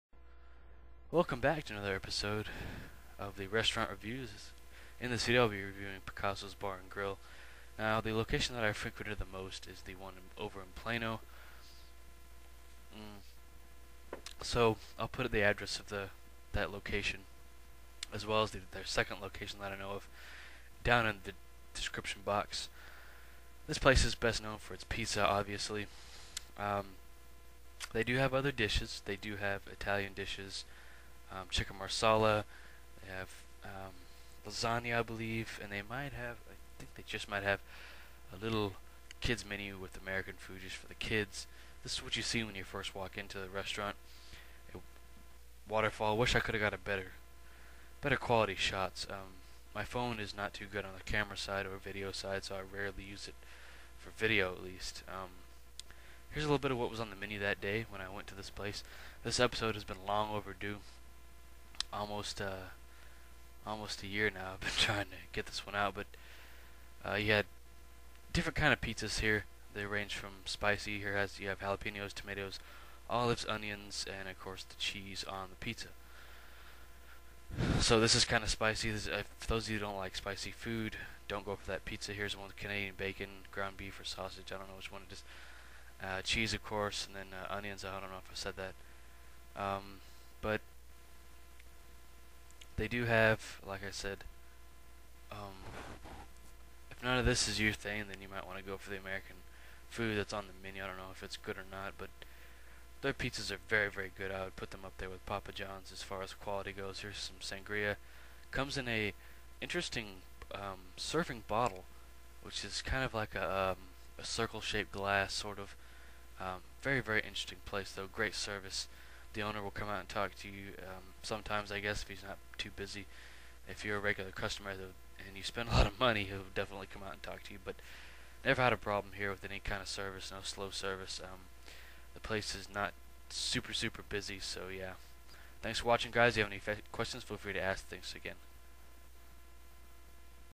Restaurant Reviews: Picassos's Pizza and Grill
Picasso's Pizza and Grill have three restaurant locations in  Dallas,Texas. The one that I frequent near the Bent Tree estates. Second location Lake Highlands 7215 Skillman St. # 300 Dallas,Tx 75231.Third location Preston Hollow 12300 Inwood Rd.